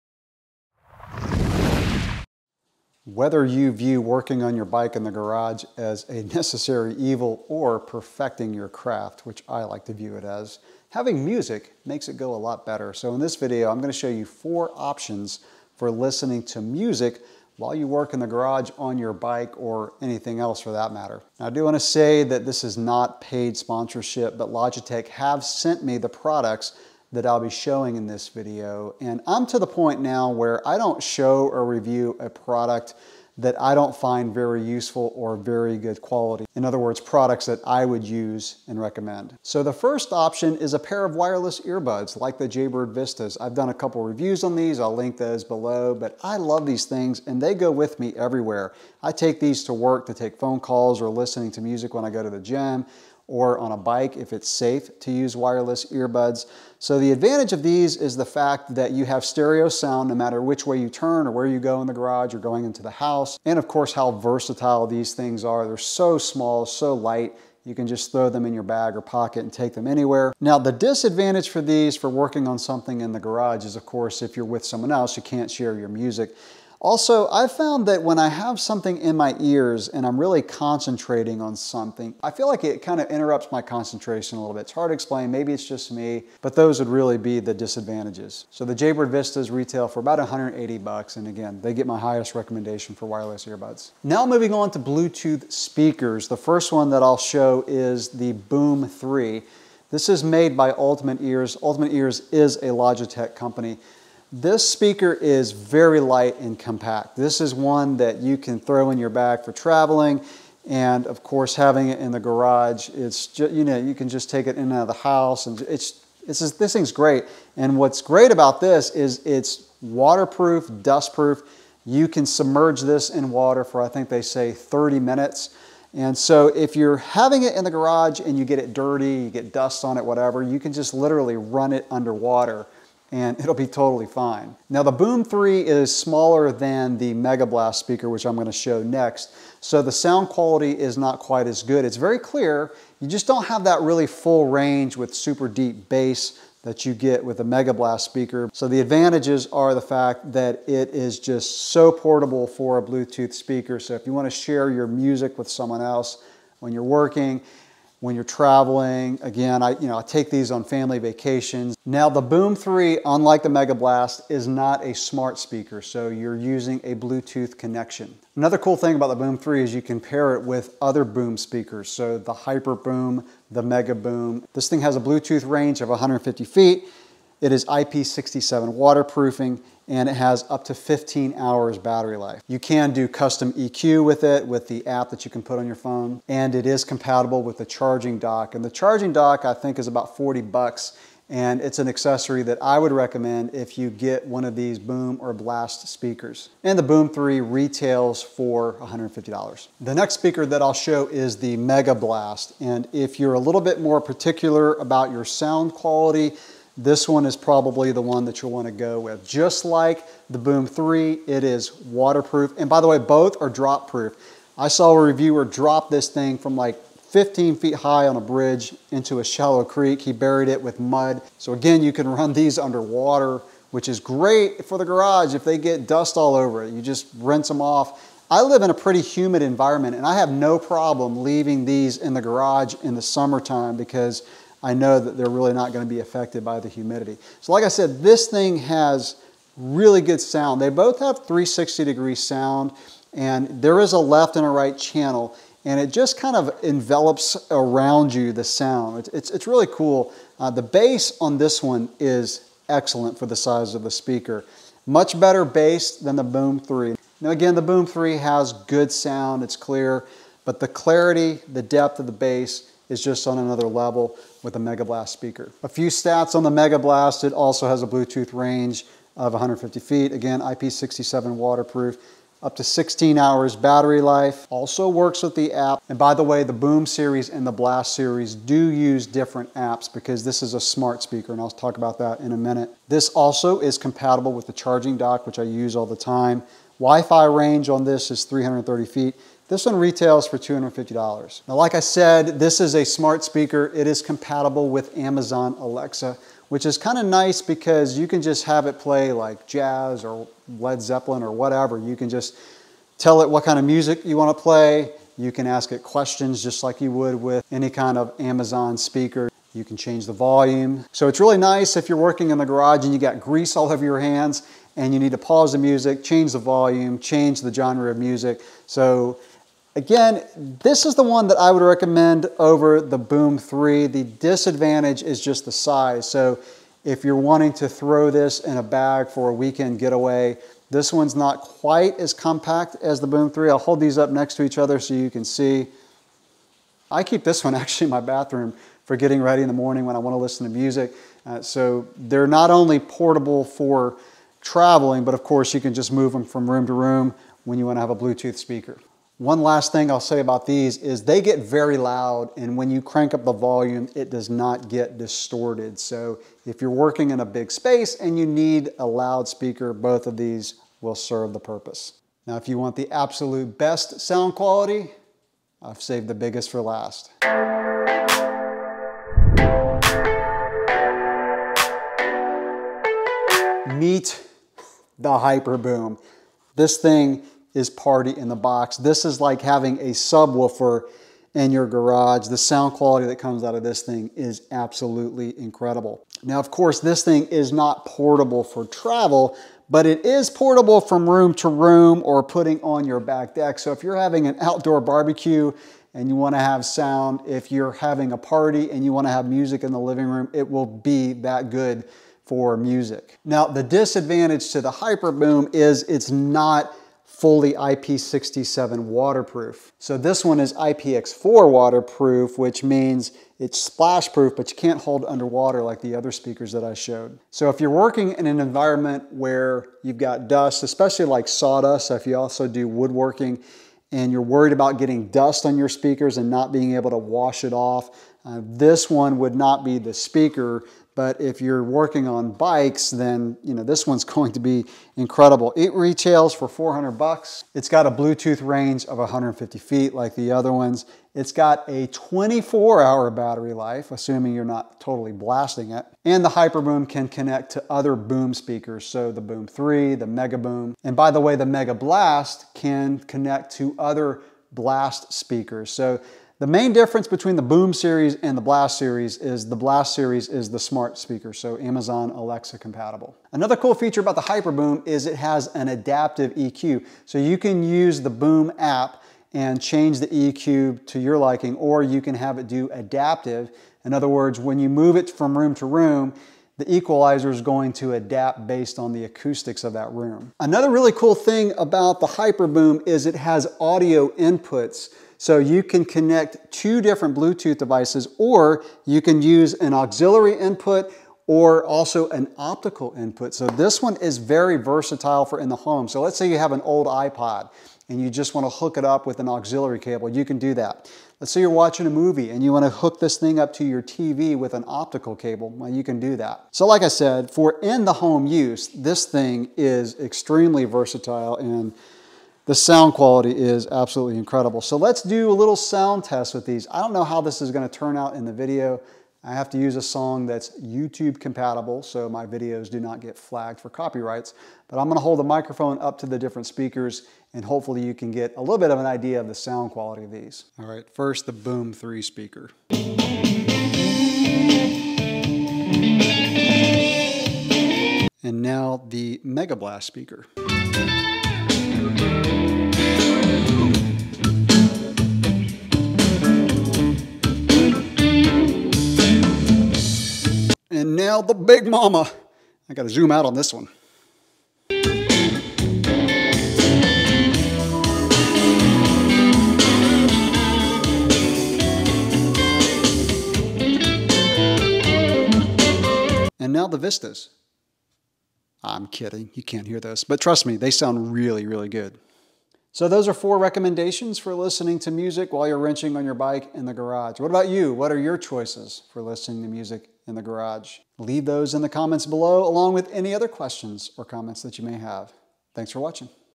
Music Options for Your Garage/Work Space
Like listening to music while wrenching on your bike in the garage? Here are 4 options.